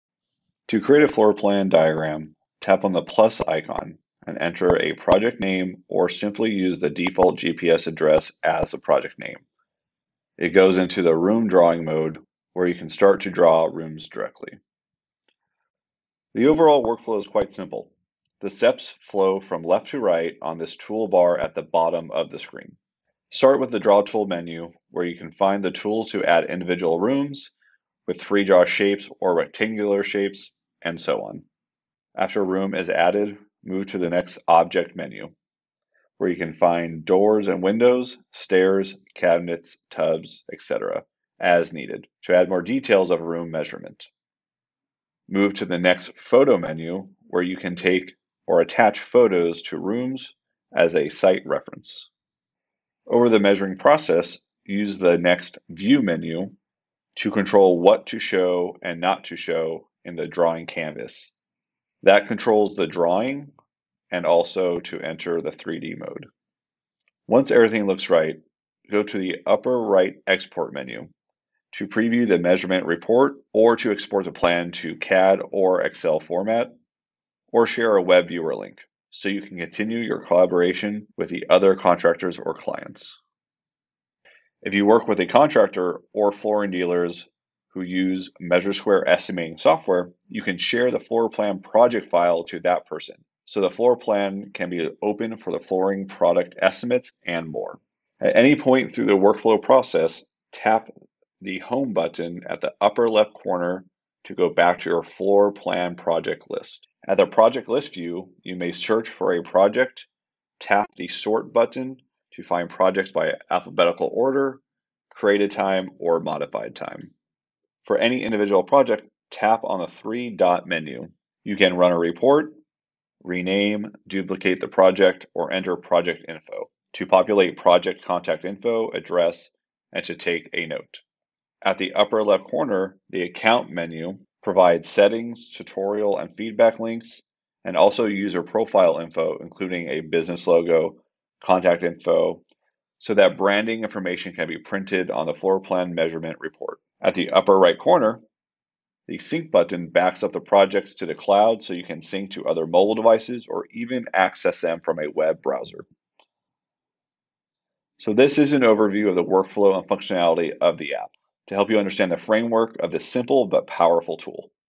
2. RenoPlan; Create a Diagram
Create a floor plan diagram easily by watching the following video.

This will walk you through the overall workflow and tools;
1.Draw
2.Objects (doors, windows, stairs, cabinets, tubs, etc)
3. Photos with markups
4.Views (2D & 3D)
5.Exports & Reports
6.Account and Syncing to the Cloud